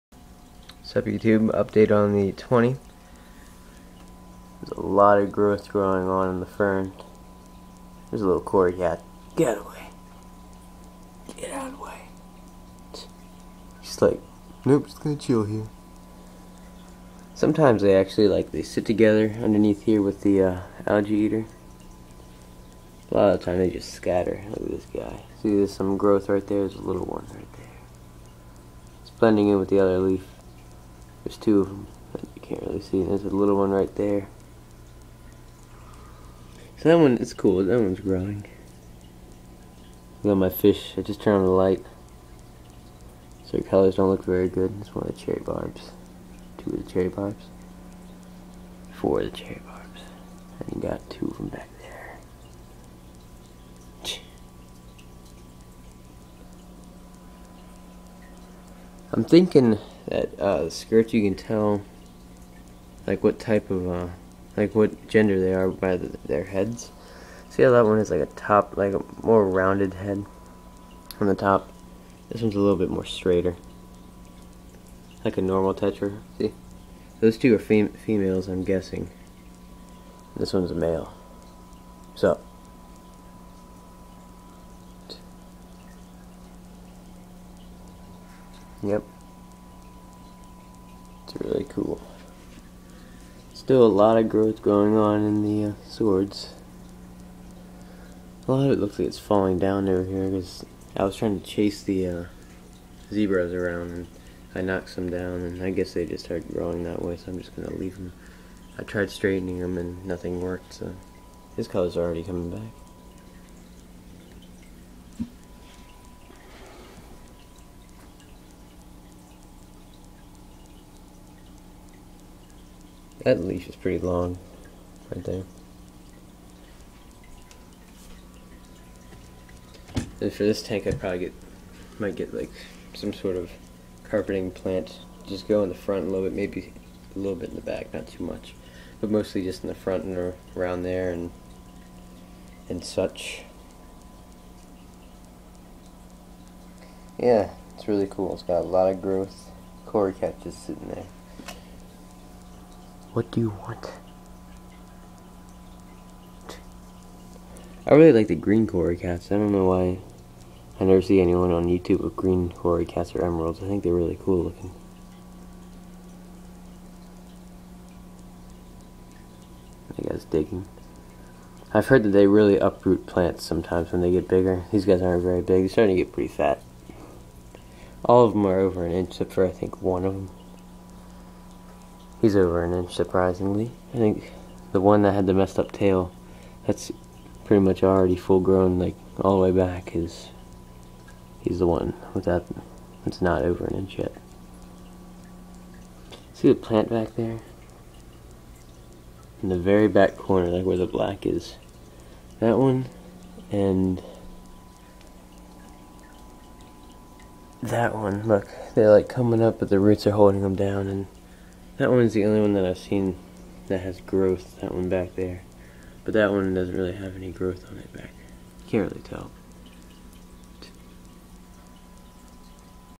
Update on 20 Gallon High
Just showing the new growth in the fern plant and swords